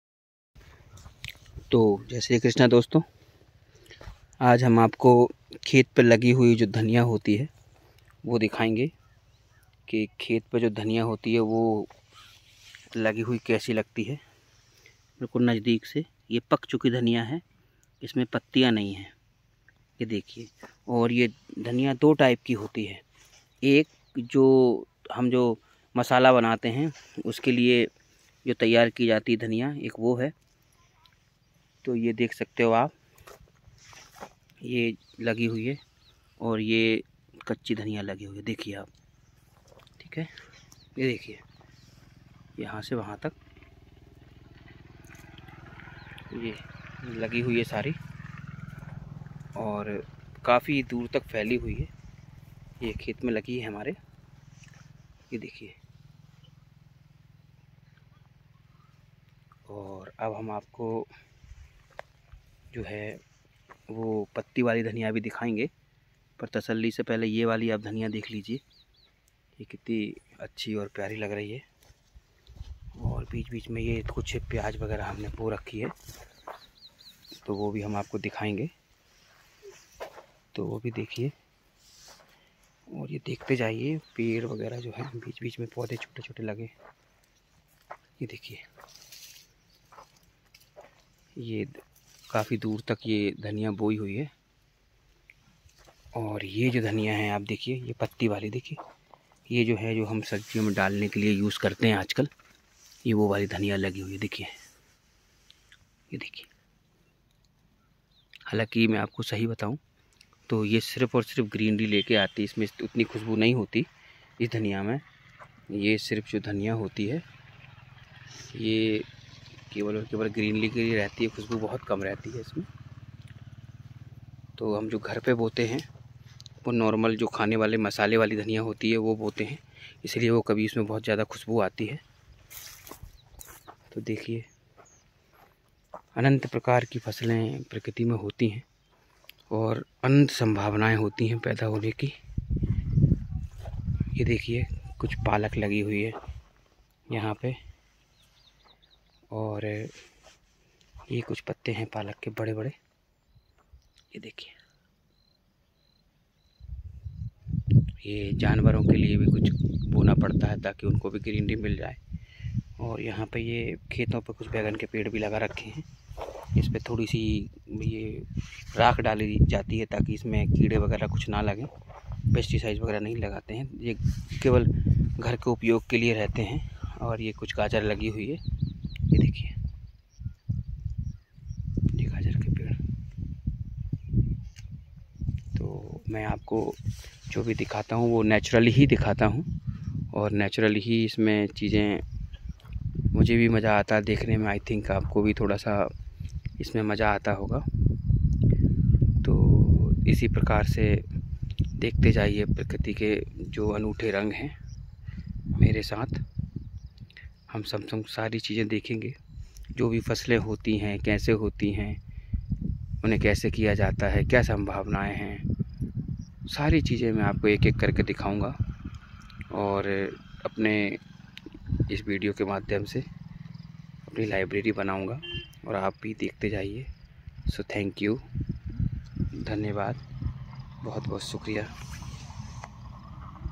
खेत पर लगी हुई धनिया, बैगन,पालक और प्याज की फसल कैसी दिखाई देती हैं
इस वीडियो में मैंने खेत पर लगी हुई धनिया पालक बैगन और अन्य सब्जियों के बारे में जानकारी दी है एवं धनिया मैं जब छोटी-छोटी धनिया लगती है तब वह किस प्रकार दिखाई देती है